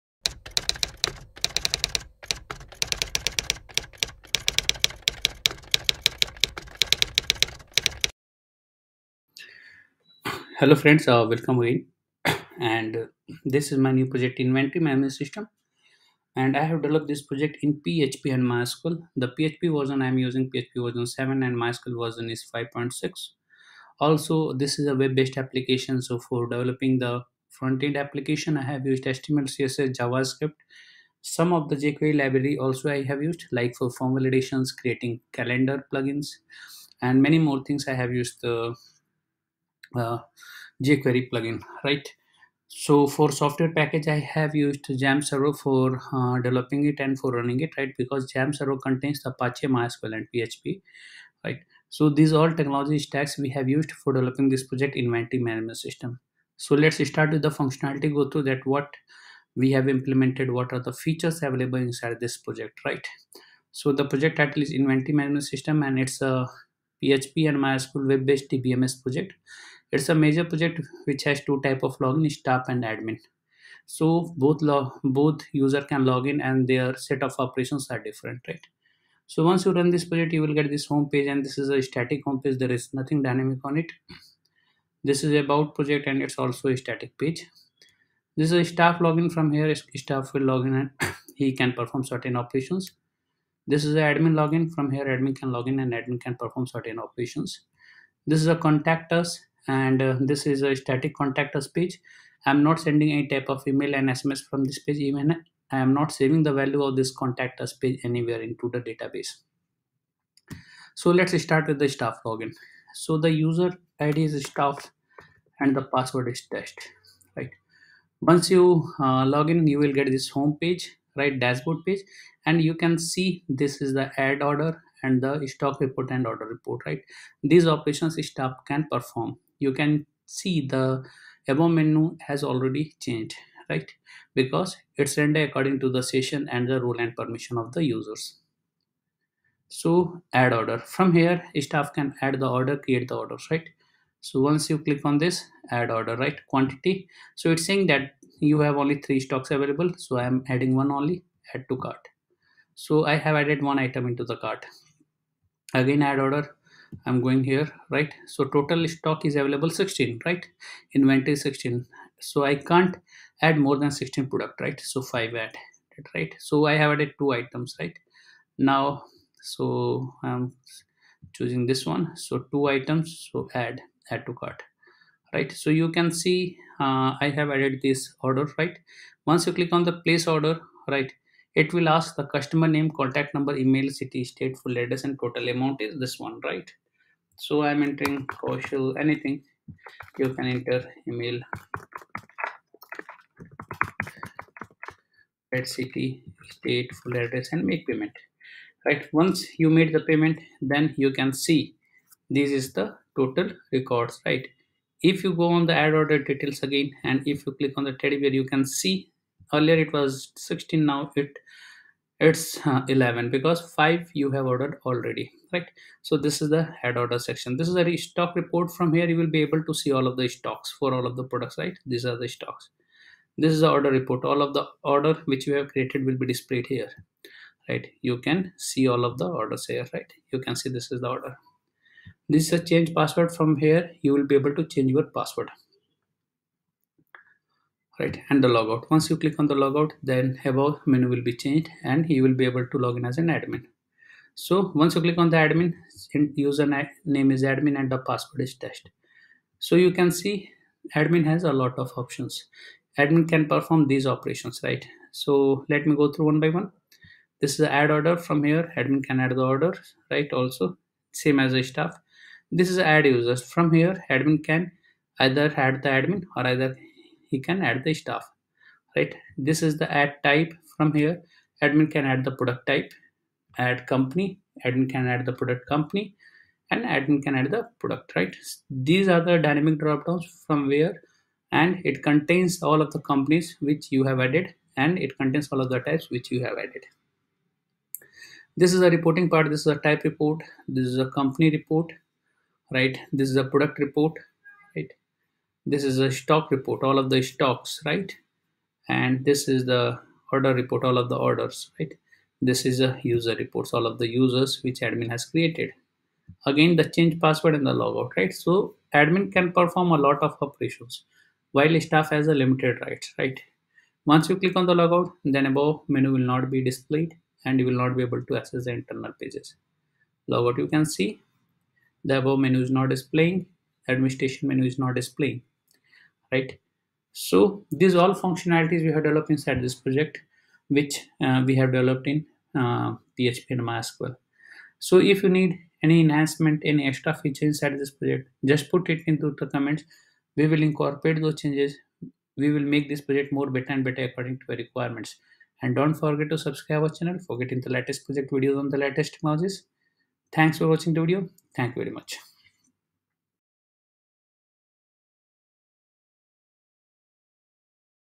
Inventory Management System | PHP and MySQL Project Source Code | PHP MySQL CRUD Project
Inventory Management System is a PHP MySQL micro service project where all APIs we have developed in PHP MySQL and all UI Inventory Management System  we have implemented in PHP.

Inventory Management System PHP and MySQL Project Tutorial
Inventory Management System Download